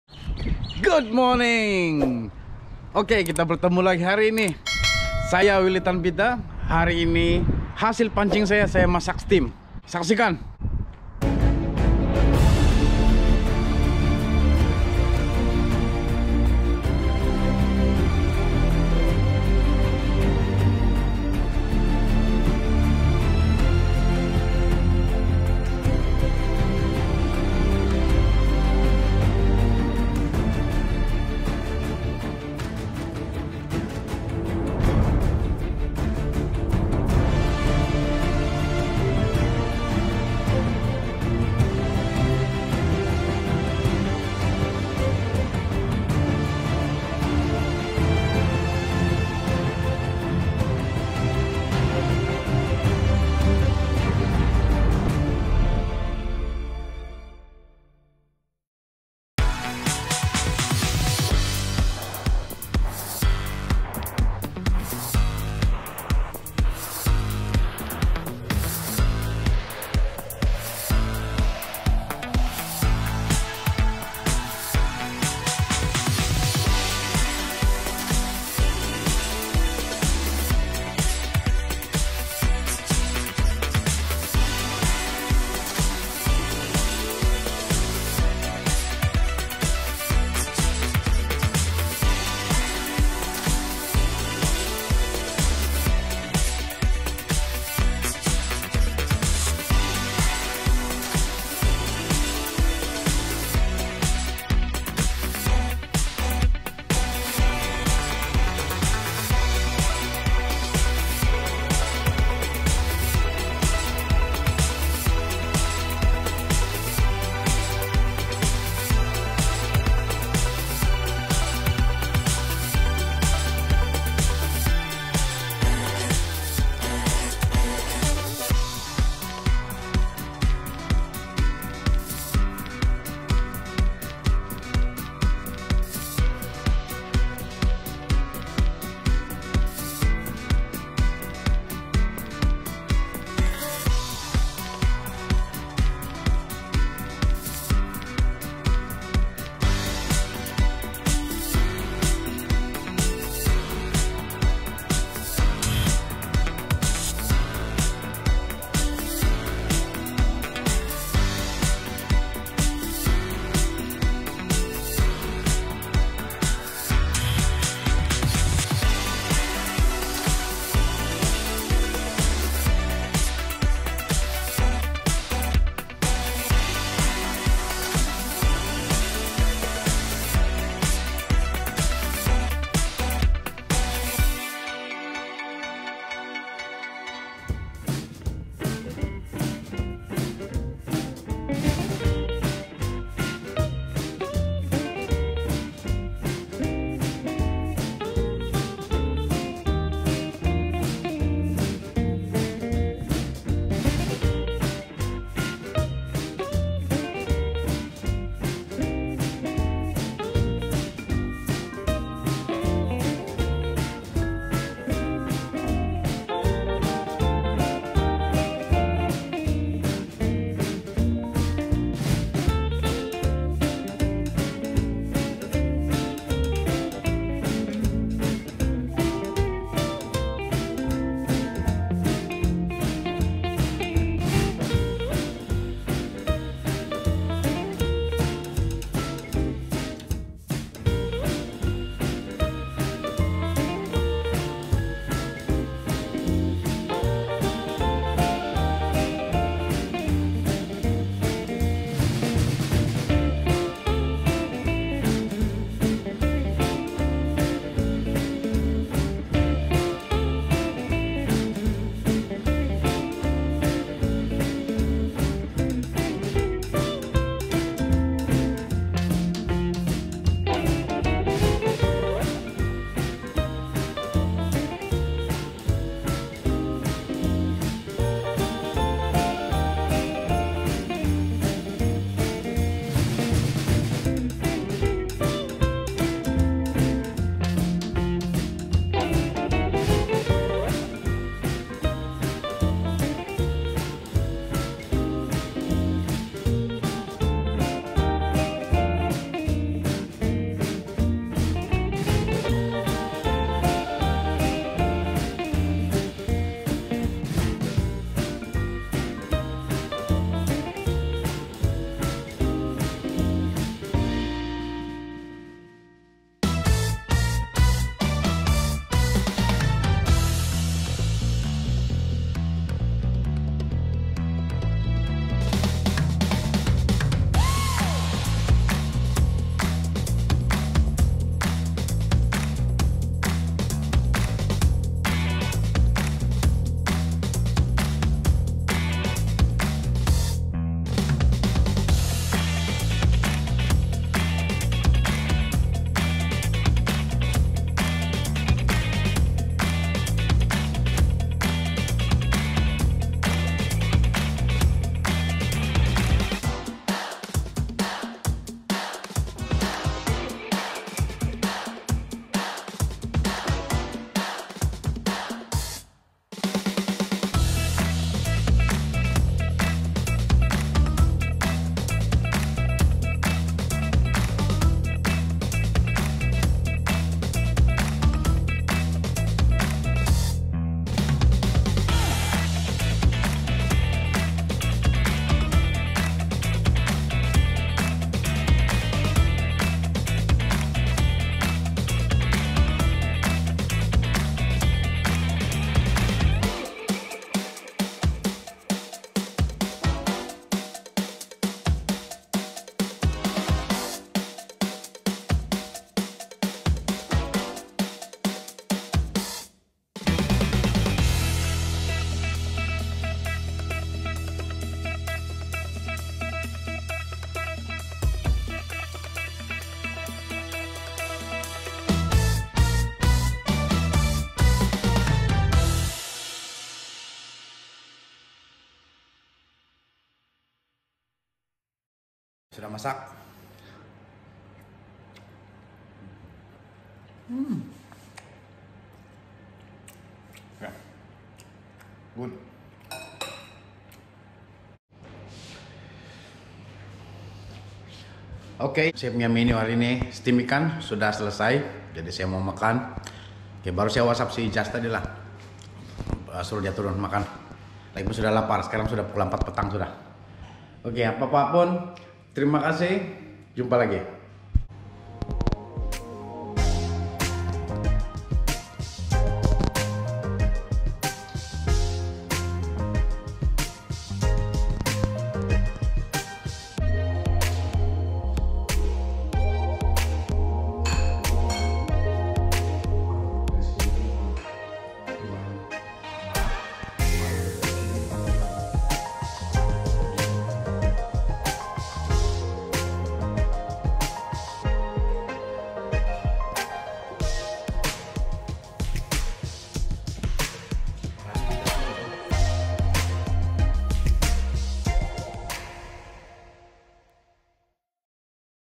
CATCH & COOK, STEAMED FISH ON ALUMINUM FOIL (Australian Salmon) Willytan Peter Style
Steamed fish mengunakan aluminum foil ialah keadaah yang baik sekiranya kita tiada periuk steamer lagi pula kos untuk steamer agak mahal... semoga ini membantu bgi yang ingin mencuba

thanks
Willytan Peter